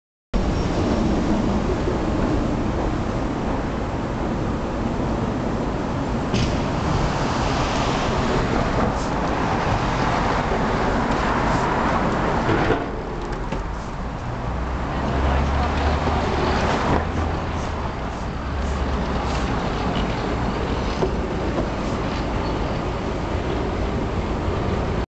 New York 7th Avenue
Just a short Movie from the 7th Avenue / 49th Street in NYC, February 2008.